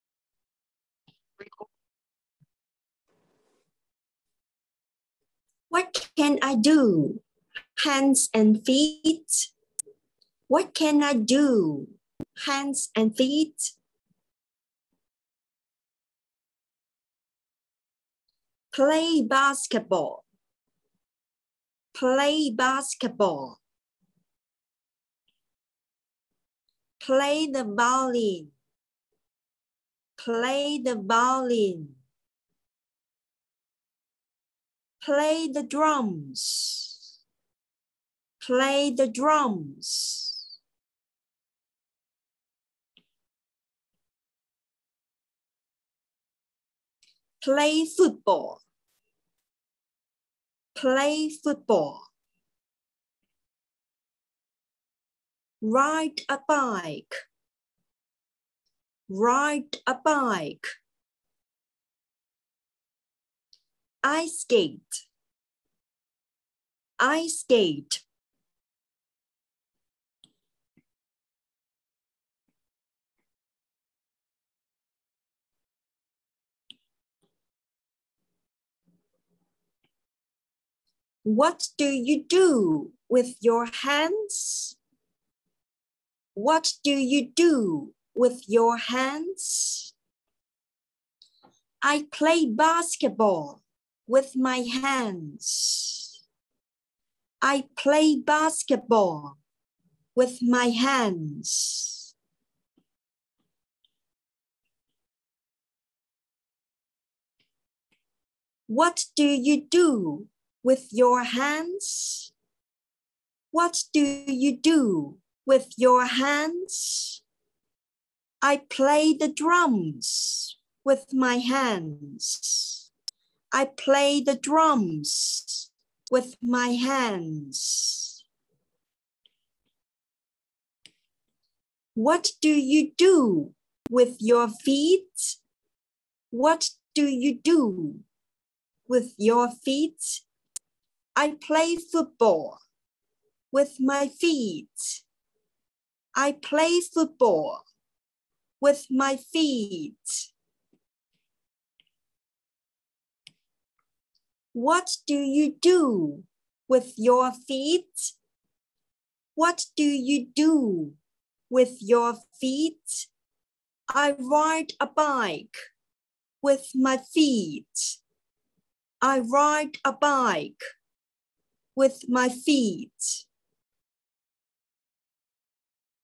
Hands and Feet - 英文生字短句會話_手和腳
1. What do you do with your hands?
I play basketball with my hands.

2. What do you do with your hands?
I play the drums with my hands.

3. What do you do with your feet?
I play football with my feet.

4. What do you do with your feet?
I ride a bike with my feet.

多朗讀是學語言必要的過程！比書寫更重要:-)
史丹利英語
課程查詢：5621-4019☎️
或